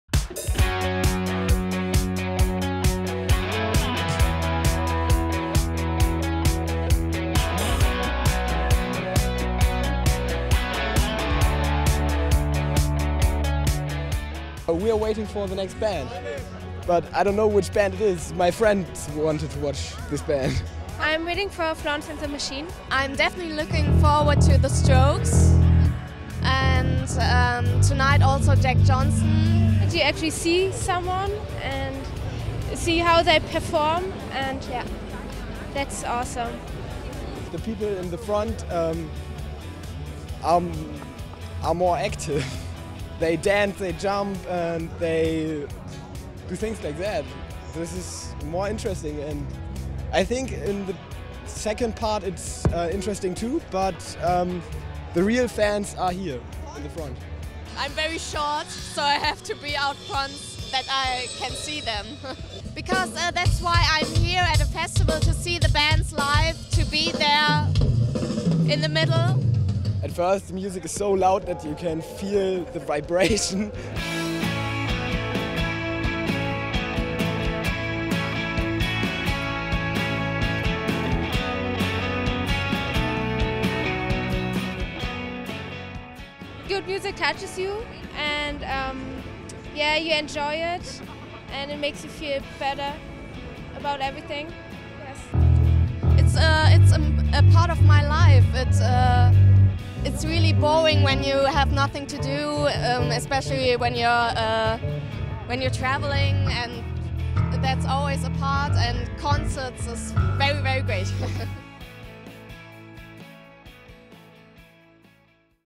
Waiting for the music at Hurricane
You have to see your favorite band in the front, there is no doubt about that.